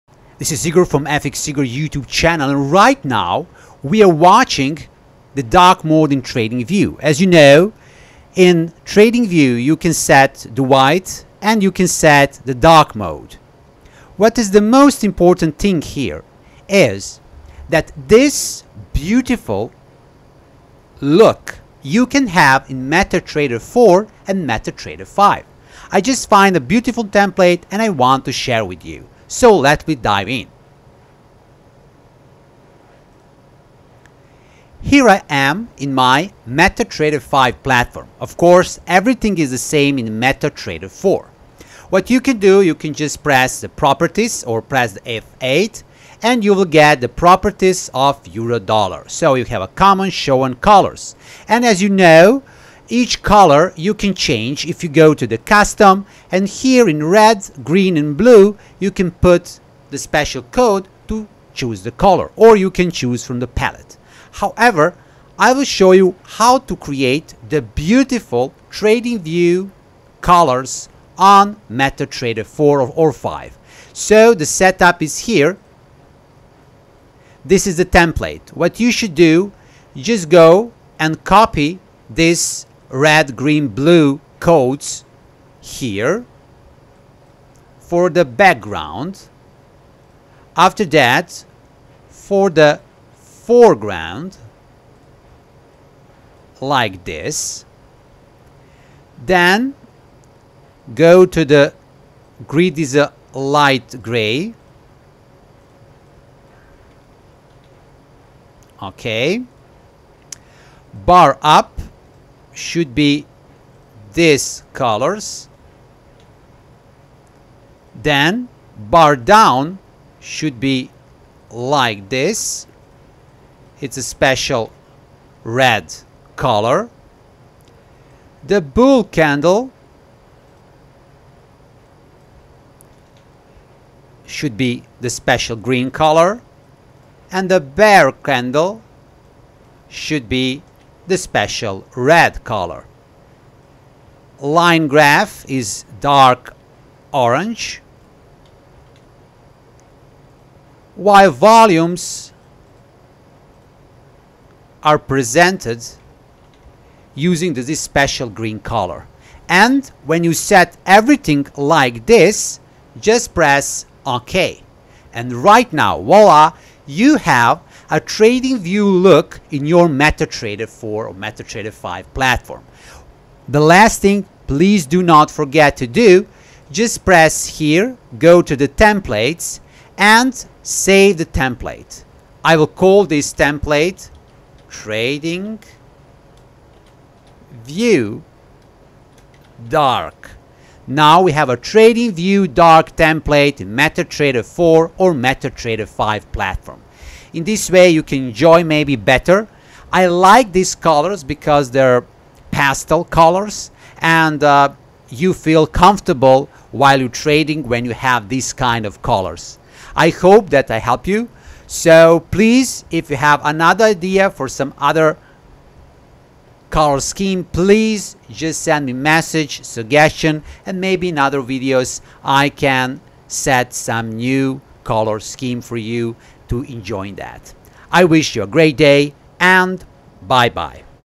Make TradingView Dark Theme Template in MT4 and MT5! - Metatrader Template Settings!
Using this setting, you can customize Metatrader 4 or Metatrader 5 to look similar to TradingView dark mode.